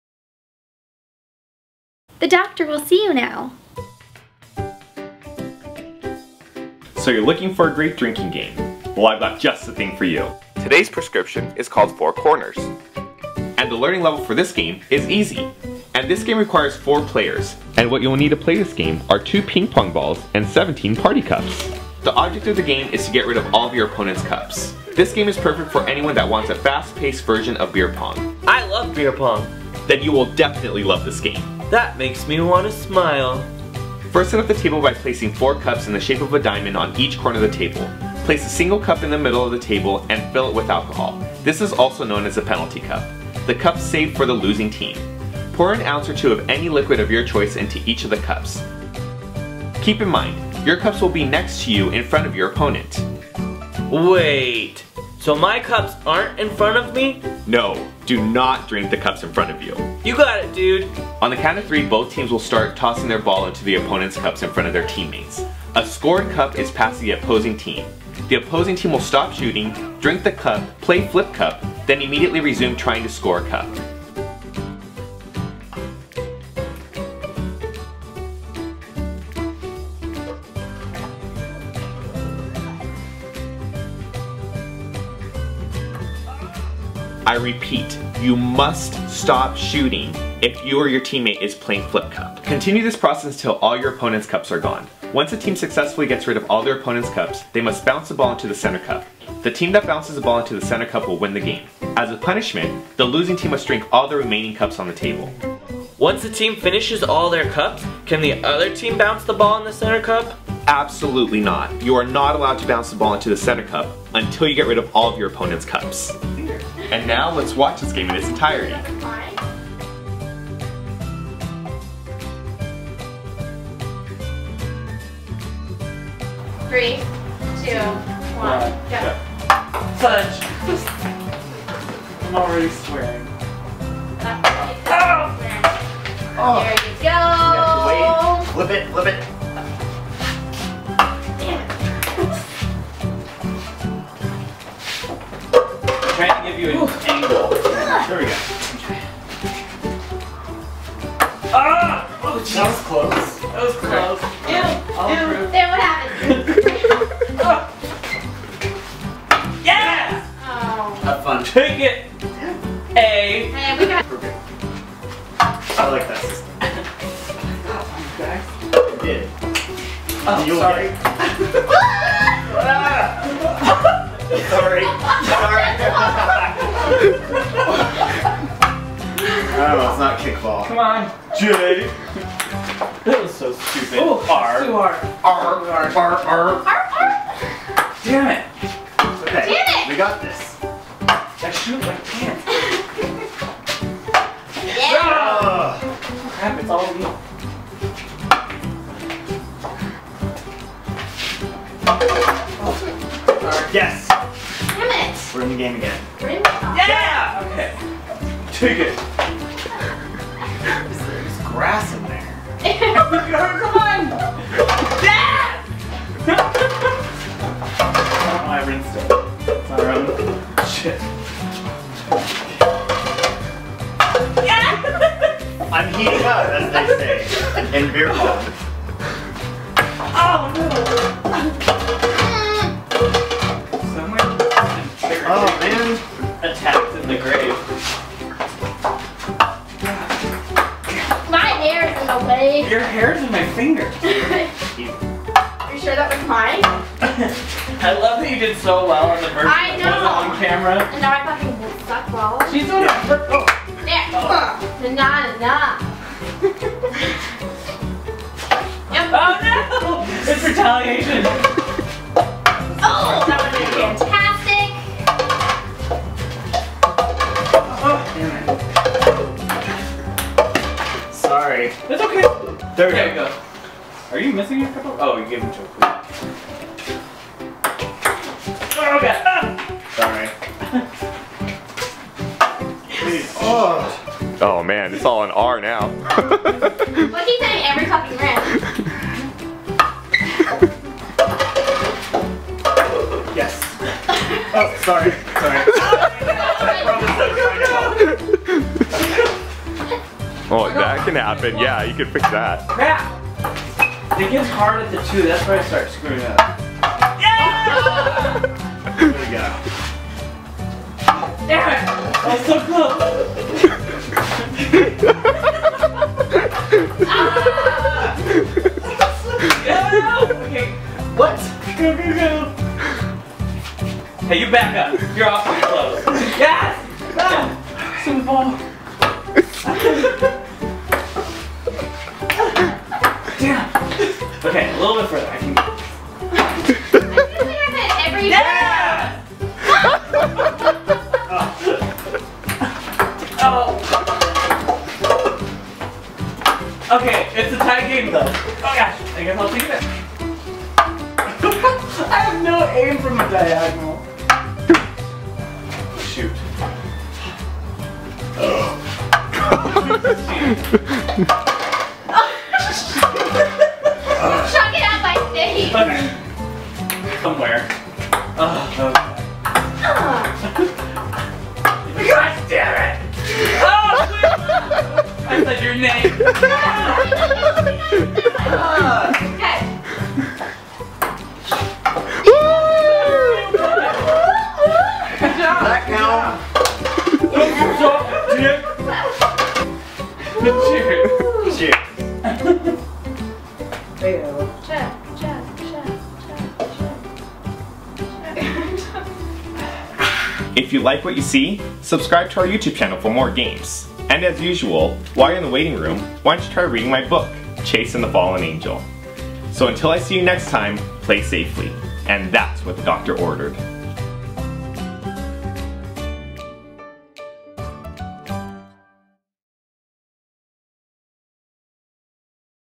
How to Play "FOUR CORNERS" by the Game Doctor (Drinking Game)
This game is a combination of Flip Cup and Beer Pong. Tons of fun to play and moves quicker than traditional Beer Pong. Get rid of your opponents cups and score the last cup in the middle to win the game.